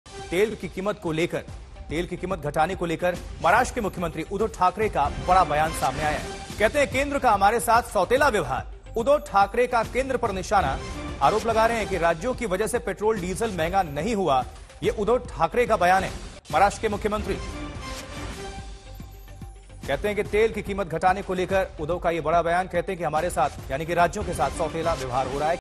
तेल कीमत घटाने को लेकर Uddhav Thackeray का बयान, कहा- हमारे साथ केंद्र का सौतेला व्यवहार | Aaj Tak
बढ़ती तेल कीमतों के लिए विपक्ष के निशाने पर आए प्रधानमंत्री नरेंद्र मोदी आज पलटवार के मूड में दिखे. मौका तो कोरोना पर मुख्यमंत्रियों की मीटिंग का था, लेकिन पीएम मोदी ने न सिर्फ पेट्रोल-डीजल के बढ़े हुए दाम का जिक्र छेड़ा बल्कि इस मामले में गेंद भी विपक्षी दलों के मुख्यमंत्रियों के पाले में डाल दी. इसपर अब उद्धव ठाकरे का बयान सामने आया है.